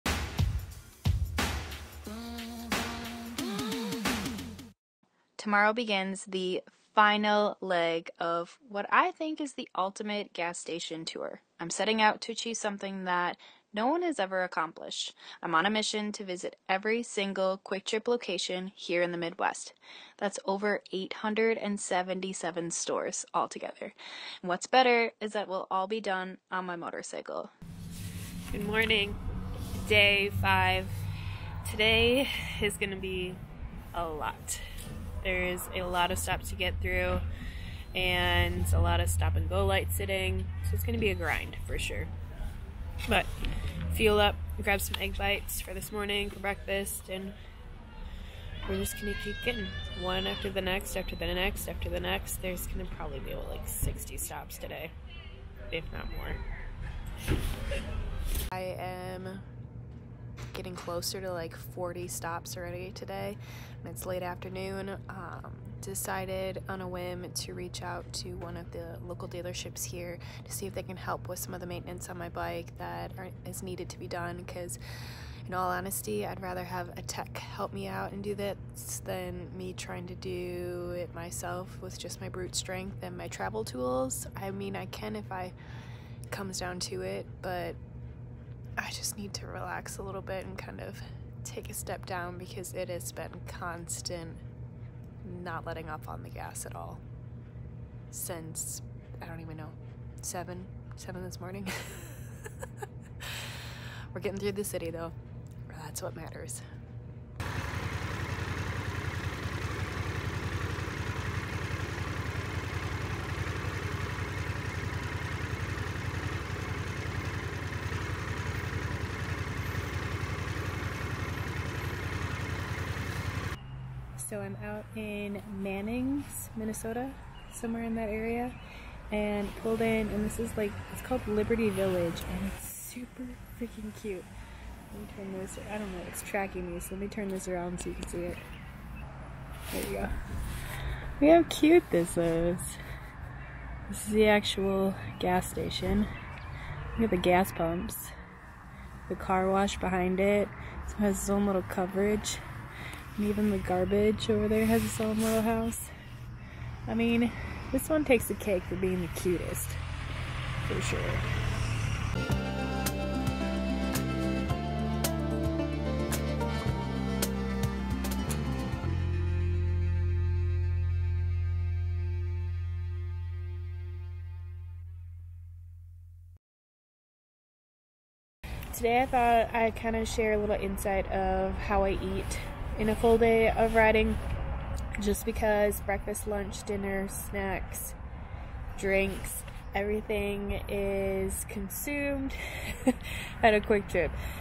SO MANY CITY STOPS - Riding to EVERY Kwik Trip Gas Station - Final Leg (Day 5)
Welcome to Day 5 of riding to all of the Kwik Trip locations throughout the Midwest (there's a total of 877 of them), and this is my final leg. I am heading through the St. Paul/Minneapolis city area, and there are so many stops today.

MY RIDING SETUP + FAVS: